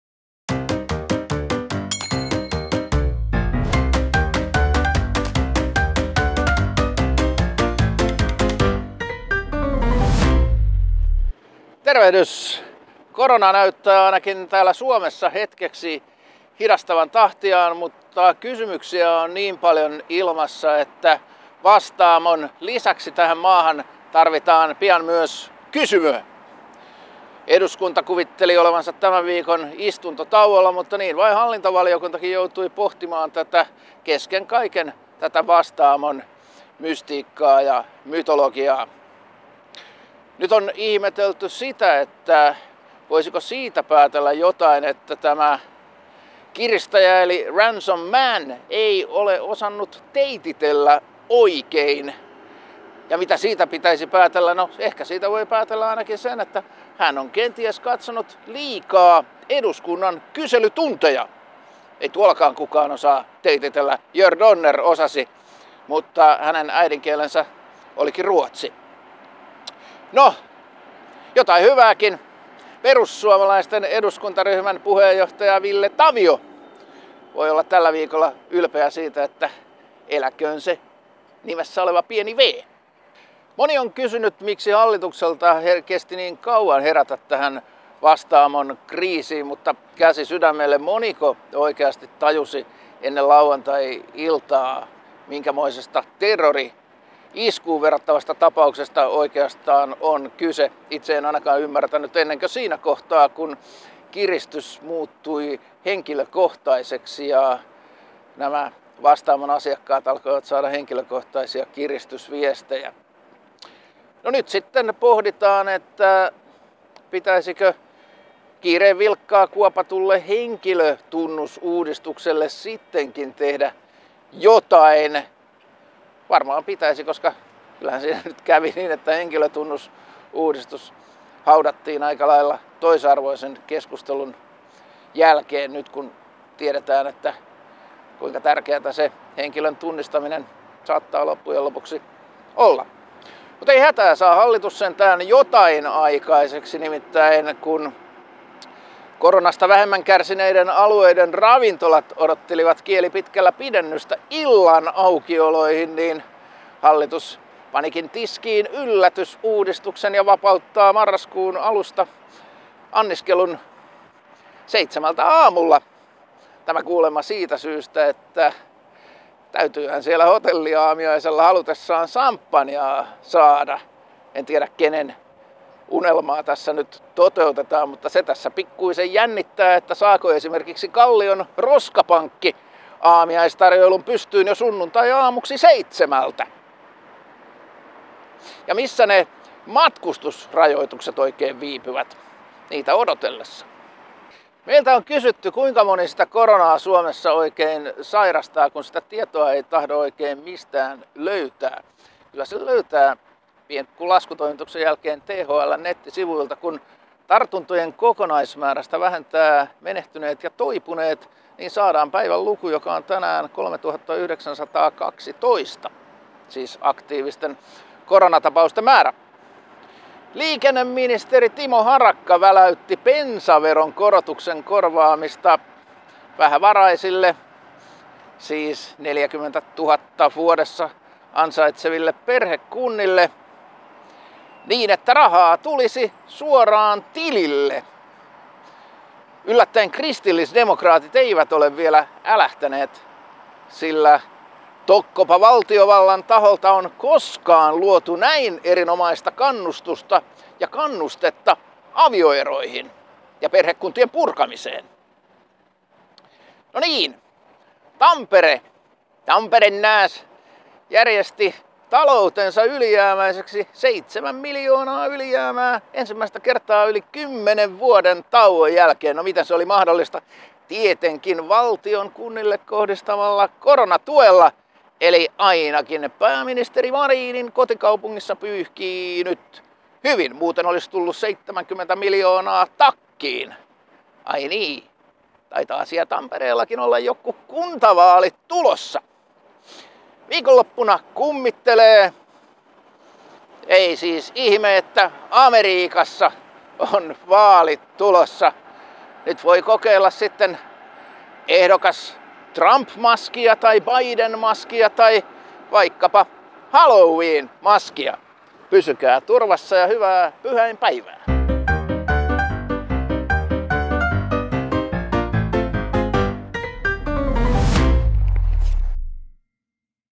KorkkiTV: Anniskelua aamuseitsemältä
Hallitus riensi koronasta kärsineen ravintola-alan avuksi ja vapautti aikaiset samppanja-aamiaiset.